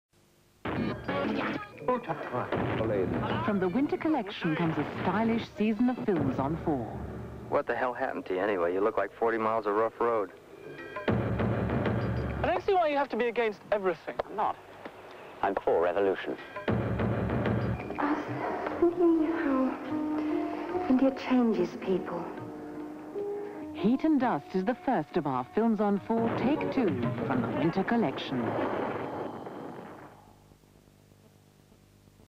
Winter Collection - Channel 4 Seasonal Identity
Something I designed whilst working at Robison Lambie-Nair in the late 80s, photography by Terence Donovan